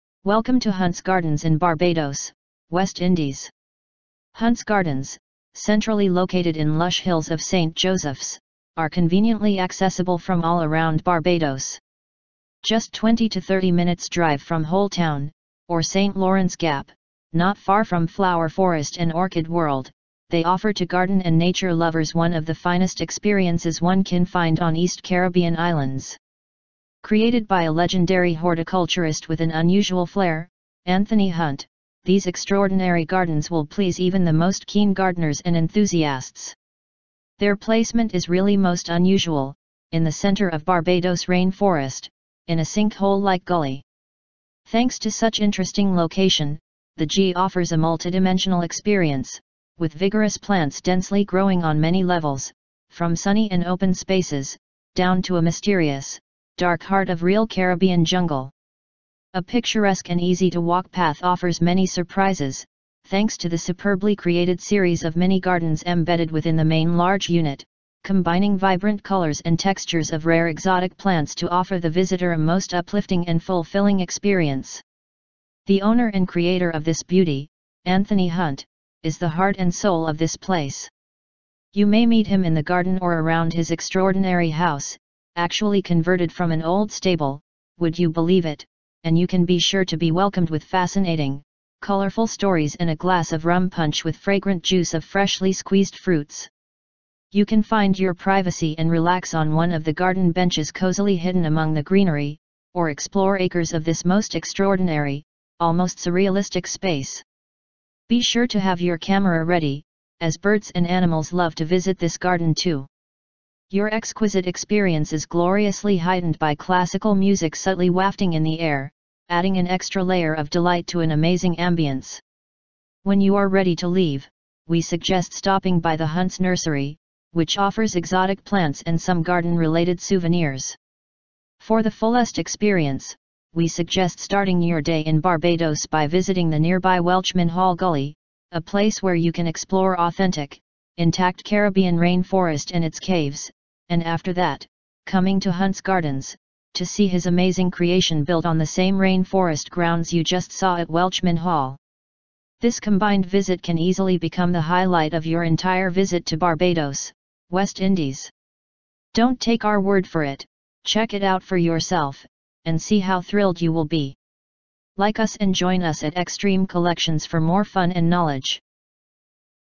Welcome to Huntes Gardens_Barbados_West Indies
Hunte's Gardens, centrally located in lush hills of St. Joseph's, are conveniently accessible from all around Barbados. Just 20 to 30 minutes' drive from Holetown, or St. Lawrence Gap, not far from Flower Forest and Orchid World, they offer to garden and nature lovers one of the finest experiences one can find on East Caribbean islands.

Created by a legendary horticulturist with an unusual flair, Anthony Hunte, these extraordinary gardens will please even the most keen gardeners and enthusiasts.
Their placement is really most unusual - in the center of Barbados' rain forest, in a sink-hole-like gully. Thanks to such interesting location, the g offers a multidimensional experience, with vigorous plants densely growing on many levels - from sunny and open spaces, down to a mysterious, dark heart of real Caribbean jungle.
A picturesque and easy-to-walk path offers many surprises, thanks to the superbly created series of mini-gardens embedded within the main large unit, combining vibrant colours and textures of rare exotic plants to offer the visitor a most uplifting and fulfilling experience.

The owner and creator of this beauty, Anthony Hunte, is the heart and soul of this place. You may meet him in the garden or around his extraordinary house (actually converted from an old stable, would you believe it!), and you can be sure to be welcomed with fascinating, colorful stories and a glass of rum punch with fragrant juice of freshly squeezed fruits.

You can find your privacy and relax on one of the garden benches cosily hidden among the greenery, or explore acres of this most extraordinary, almost surrealistic space.

Be sure to have your camera ready, as birds and animals love to visit this garden too!
Your exquisite experience is gloriously heightened by classical music subtly wafting in the air, adding an extra layer of delight to an amazing ambiance.

When you are ready to leave, we suggest stopping by the Hunte's Nursery, which offers exotic plants and some garden related souvenirs.

For the fullest experience, we suggest starting your day in Barbados by visiting the nearby Welchman Hall Gully - a place where you can explore authentic, intact Caribbean rain forest and its caves - and after that - coming to Hunte's Gardens, to see his amazing creation built on the same rain forest grounds you just saw at Welchman Hall.

This combined visit can easily become the highlight of your entire visit to Barbados, West Indies.
Don't take our word for it - check it out for yourself, and see how thrilled you will be!